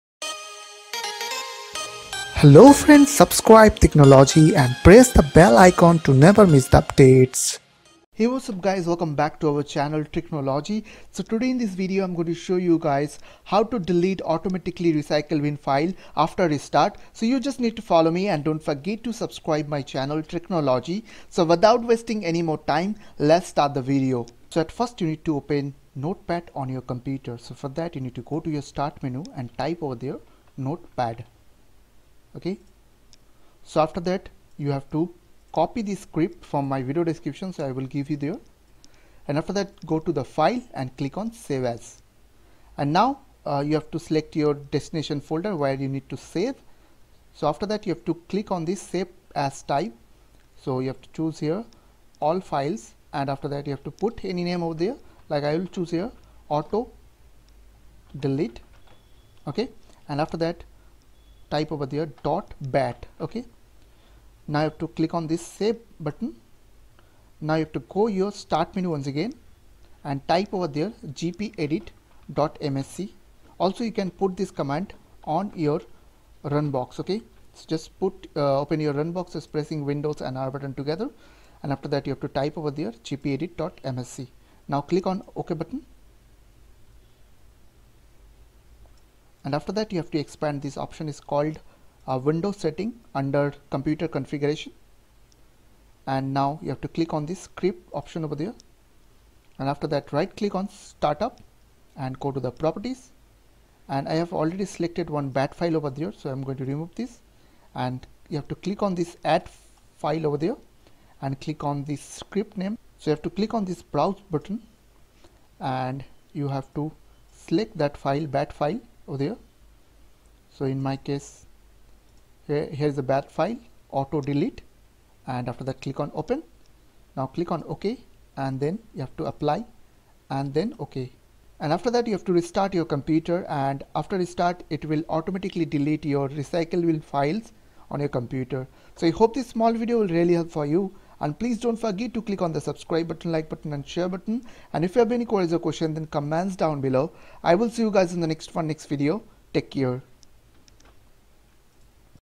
How To Empty Automatically Recycle Bin After Shutting Down Your Windows PC
In this video, I will show you guys how to automatically delete your computer recycle bin files after shutdown or restart your pc or laptop.

bat command : rd /s /q C:\$Recycle.bin

run command: gpedit.msc

1. Mic
2. Microphone Stand
3. Headphone
4. Phone
5. Strip LED Light